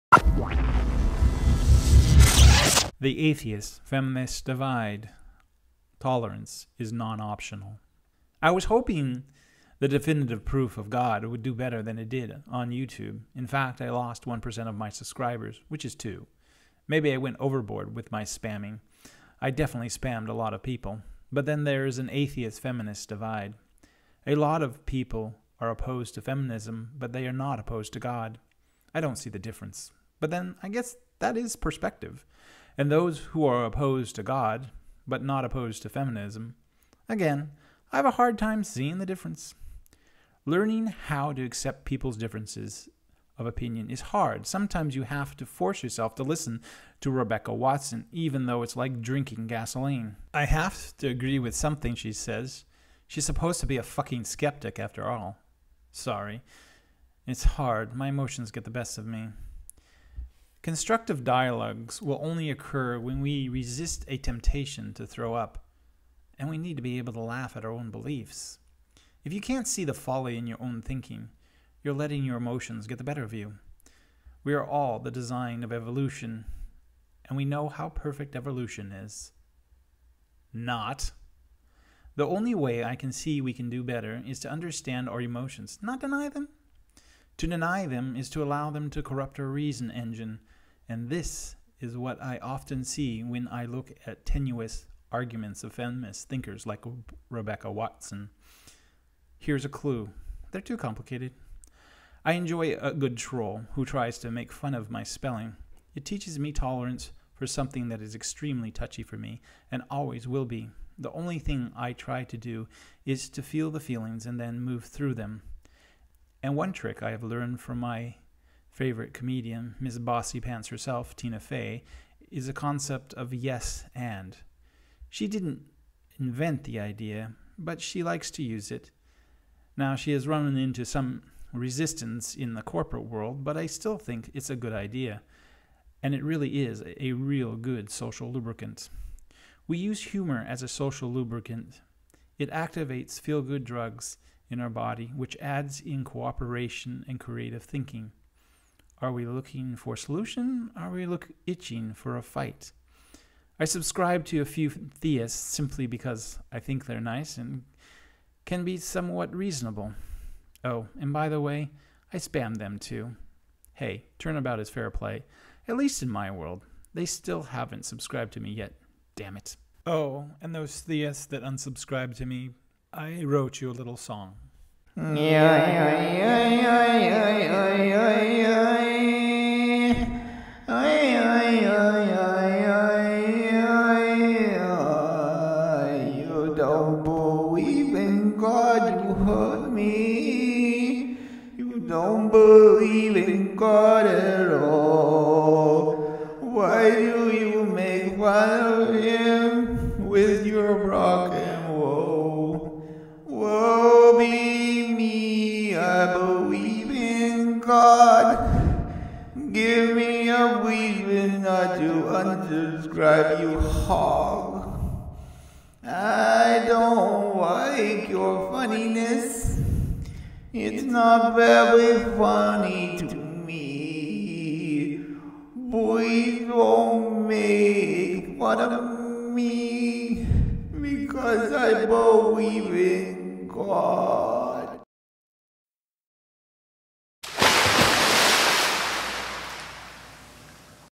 The atheists feminist divide, tolerance is non optional.
If you make it to the end there is a Song for believers in god. About a minute from the end. Skip ahead if you like that kinda thing, and the rest of my video boars you to tears.

I was hoping definitive proof of god, would do better than it did, on YouTube in fact I lost %1 of my subscribers, which is two.  Maybe I went overboard with my spamming, I deffently spammed a lot of people.    But then their is an Atheist Feminist divide.   A lot of people are opposed to Feminism, but they are not opposed to god.  I don’t see the difference, but then I guess that is perspective, and those who are opposed to god, but not opposed to feminism, again I have a hard time seeing the difference.

 Learning how to excepte peoples difference of opinion is hard, sometimes you have to force yourself to listen to Rebecca Watson even these it’s like drinking gasoline.  I have to agree with something she suppose to be a fucking skeptic after all!  Sorry it’s hard, my emotions they get the best of me.

 Constructive dialog will only accrue when we can resist a temptation to throw up, and we need to be able to laugh at our own beliefs.

If you can’t see the folly in your own thinking you're letting your emotions get the better of you. we are all the design of evolution, and we know how perfect evolution is…. not!

 The only way I can see we can do better is to understand our emotions not deny them.  To deny them is to allow them to corrupt are reason engine, and this is what I often see when I look at the tenuous arguments of feminist thinkers like Rebecca Watson.  Here is a clue they're to complicated.

 I enjoy a good troll, who tries to make fun of my spelling, it teaches me tolerance for something that is  extremely touchy for me, and always will be.  The only thing I try to do is to feel the feeling and then move through them.  And one trick I have learned from one of my favorite comedians Miss Bossy Pants herself Tina Fey is the concepted of “Yes and”  She didn't event the idea, but she likes to use it, Now she has found resistance to this idea in the corporate world, but I still think it’s a good idea, and it really is a real good social lubricant.

 We use humor as a social lubricant it actives feel good, drugs in our body which adds in cooperation and creative thinking….  Are we looking for solution or are we itching for a fight.